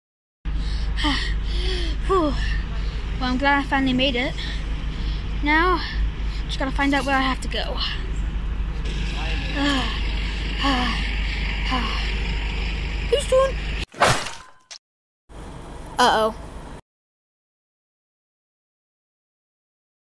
STAN'S ACCIDENT 2. (Teaser)
When Stan finds himself stuck in his accident with Kenny once again, he must find a way to escape prison with the help of his best friend, Kyle.

This film will be available both on this channel and Lil Petey's Plush Films.